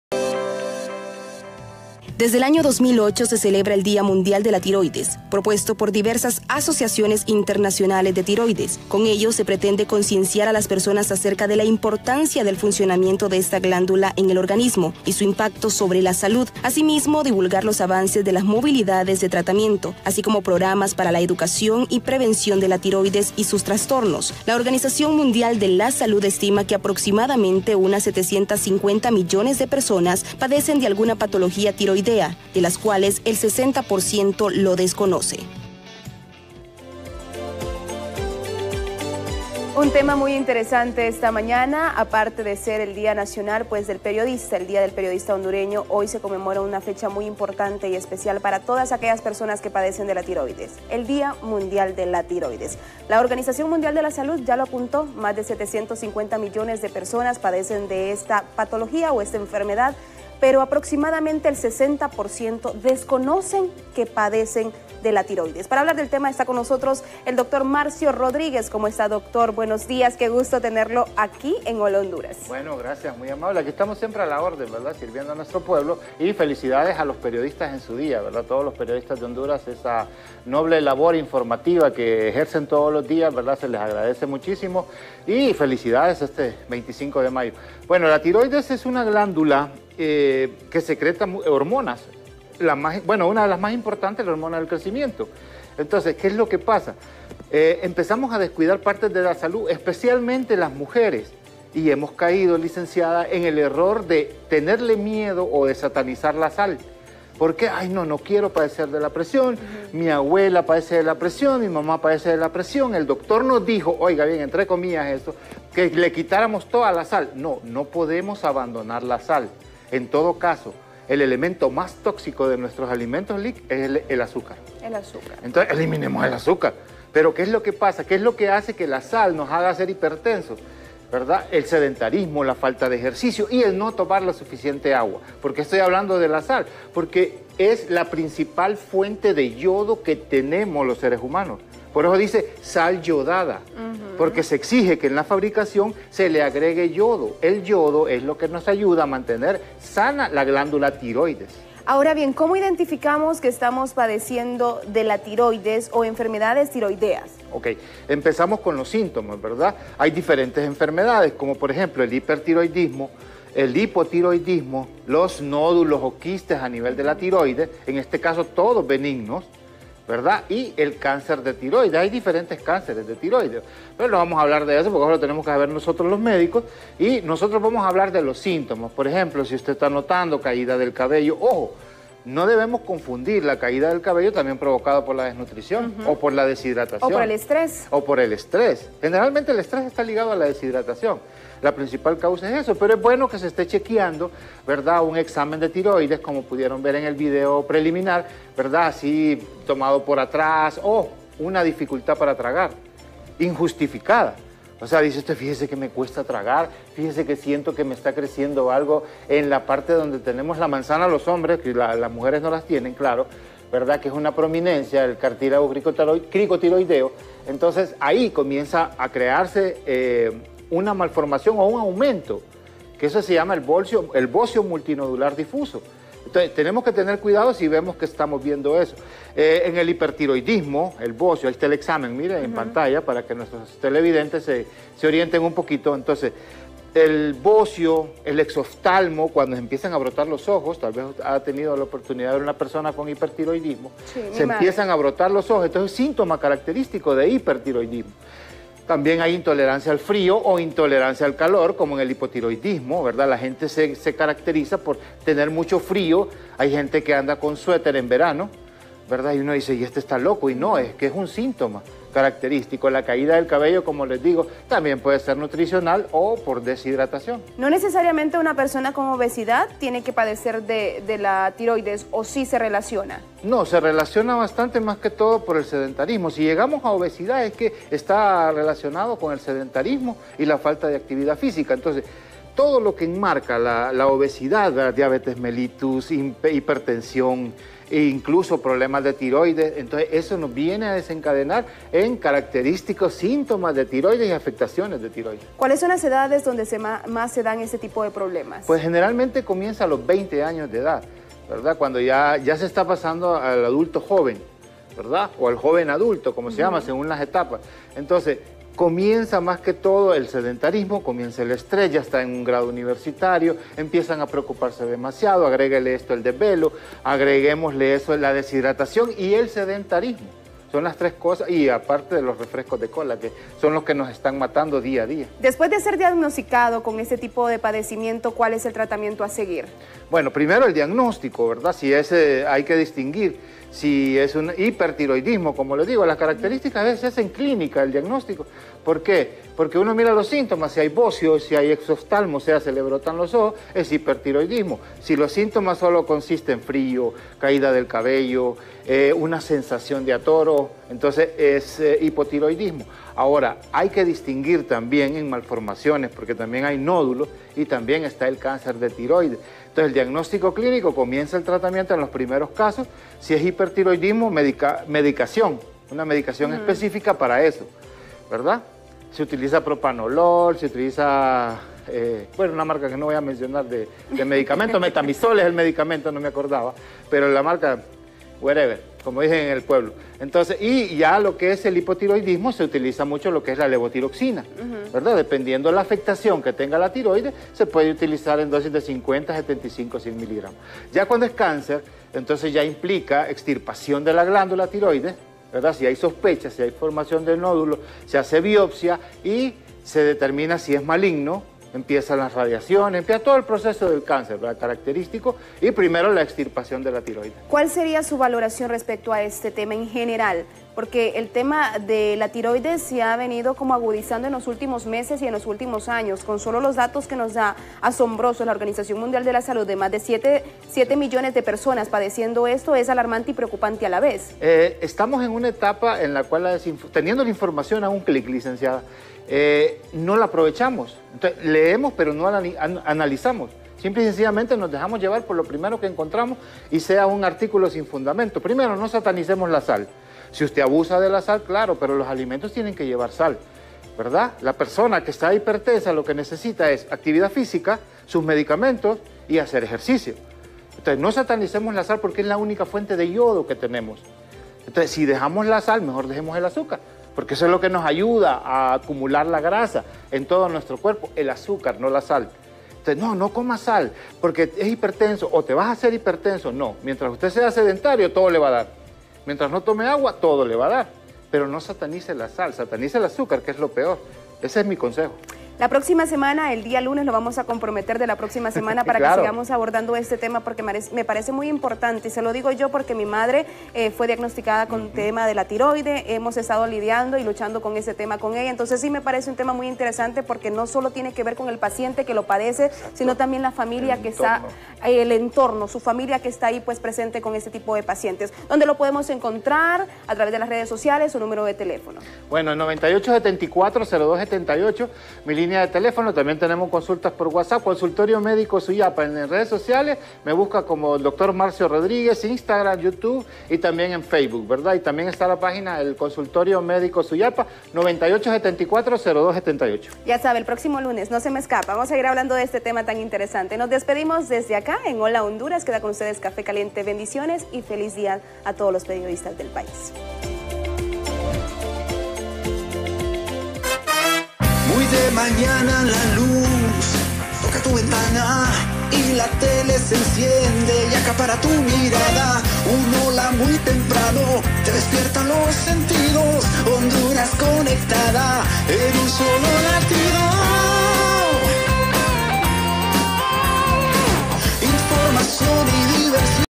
Salud y vida: Día Internacional de la tiroides
El Dr. Marcio Rodríguez, nos habló sobre este importante tema